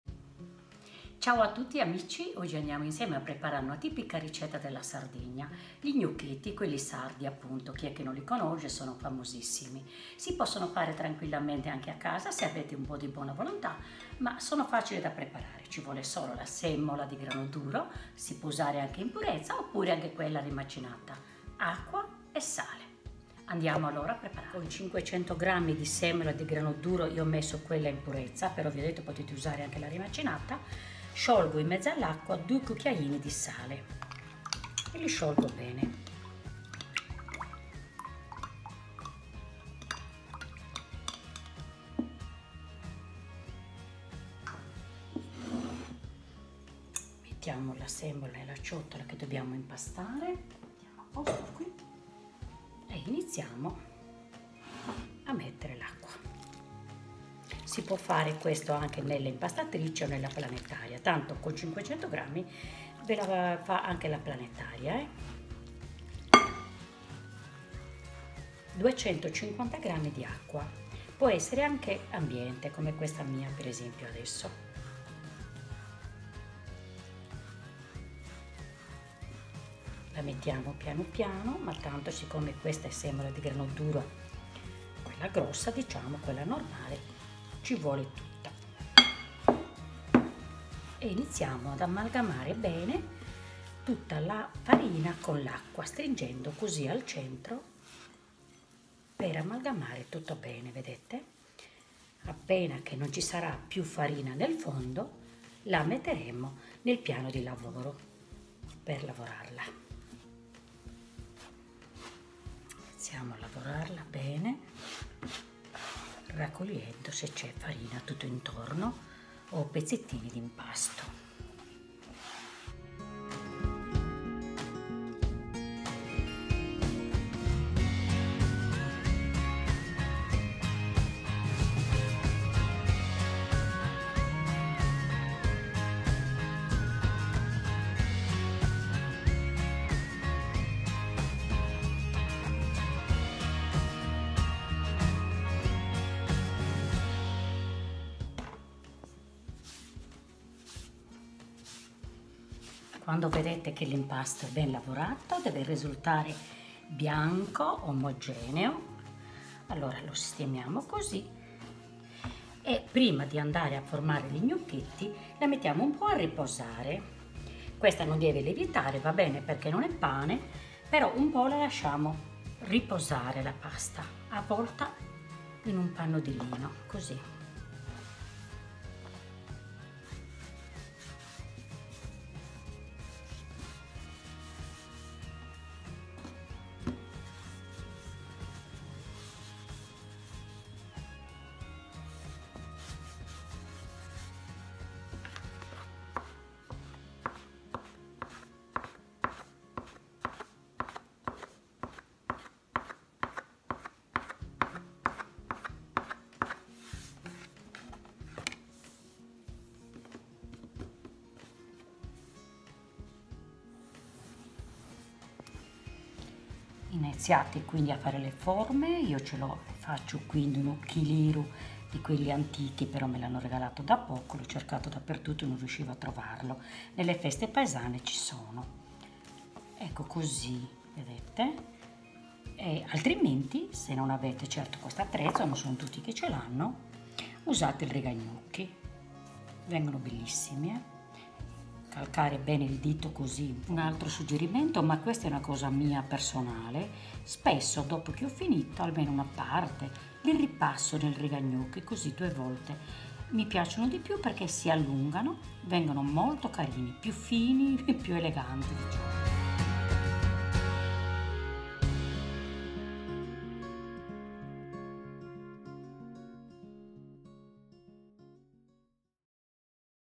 Gnocchetti sardi fatti in casa
500 grammi di semola di grano duro, 2 cucchiaini di sale, 250 mg di acqua